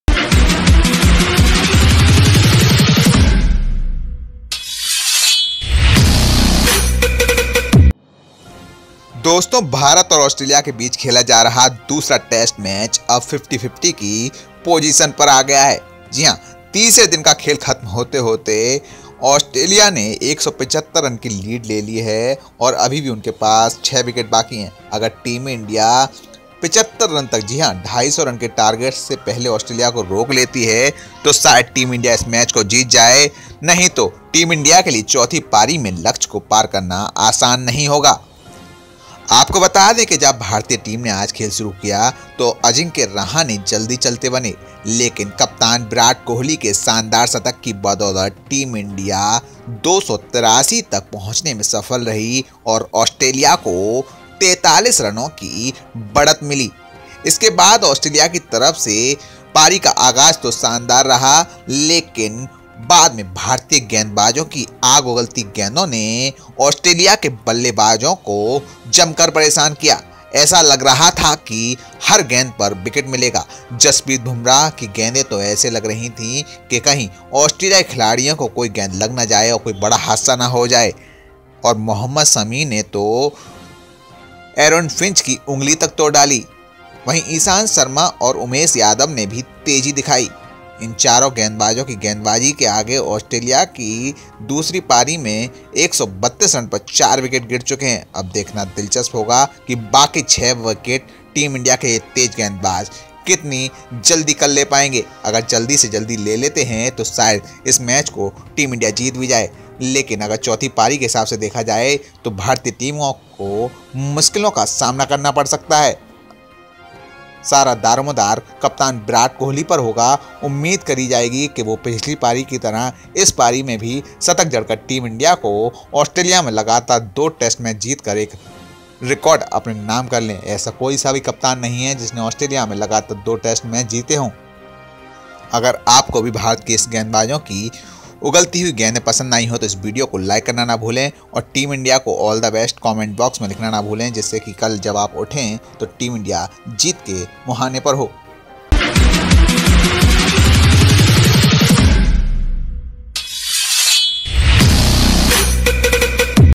Watch Full Highlights of India Vs Australia 2nd Test Day 3 | Virat Kohli Hundred | Jasprit Bumrah
Indian pacers took four wickets in the final session but Australia managed to stretch their lead to 175 at stumps on Day 3 of the second Test in Perth. Usman Khawaja and Travis Head strung together a 35-run partnership as Australia ended Day 3 on 132 for four. In the second innings, Australia got off to a solid start but Aaron Finch (25) was retired hurt after suffering a blow to the finger. After Finch, Marcus Harris (20), Shaun Marsh (5) and Peter Handscomb (13) departed in quick succession leaving Australia reeling at 85 for three. Khawaja and Head then showed resilience and played with patience against the Indian bowlers. But Head (19) towards the end played a poor shot and threw his wicket away. Skipper Tim Paine (8*) and Khawaja (41*) were unbeaten at the middle when umpires decided to call it a day. For India, Mohammed Shami picked two wickets while Ishant Sharma and Jasprit Bumrah clinched a wicket apiece. Earlier, Nathan Lyon produced figures of five for 67 as India were bowled out for 283 in the first innings. Skipper Virat Kohli (123) scored a terrific century but the lower middle order disappointed once again as the visitors crumbled in the post-lunch session. India lost their last four wickets for just 31 runs. Apart from Kohli, Hanuma Vihari (20) and Rishabh Pant (36) contributed with the bat on the third day but India still fell 43 runs short of Australia's first innings total. Apart from Lyon, Mitchell Starc and Josh Hazlewood picked two wickets apiece while Pat Cummins took the crucial wicket of Virat Kohli.